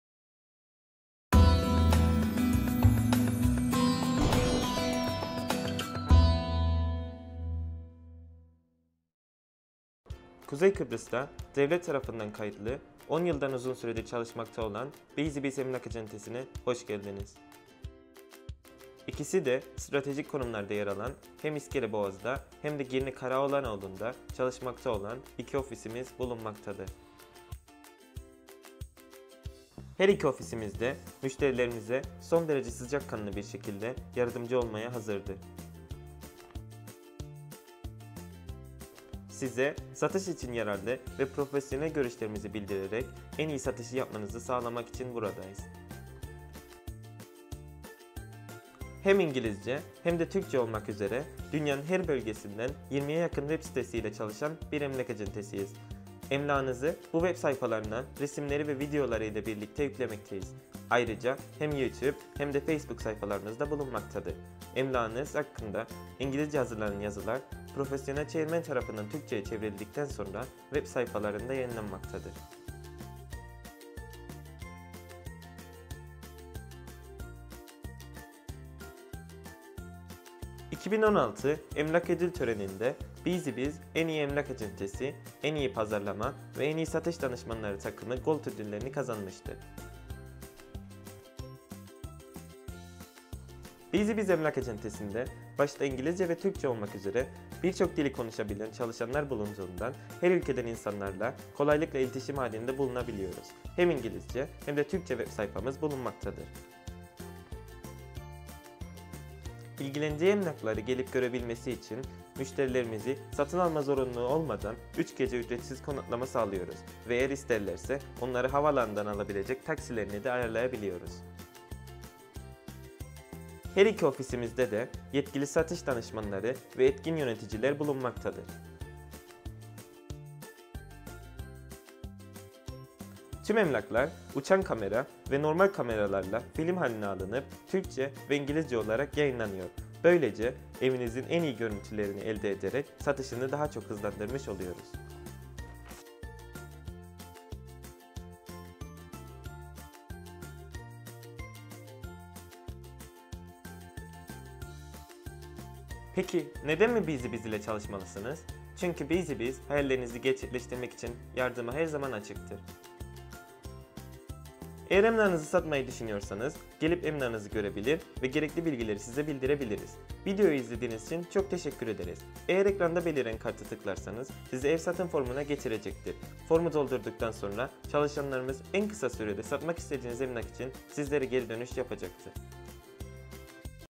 Busy Bees Emlak Acentesi - Emlağınızı Satmak mı İstiyorsunuz?
Emlağınızı Satmak mı İstiyorsunuz?

Busy Bees Emlak Acentesi ile arazi veya mülkünüzü en iyi fiyat ile hızlıca satabilirsiniz.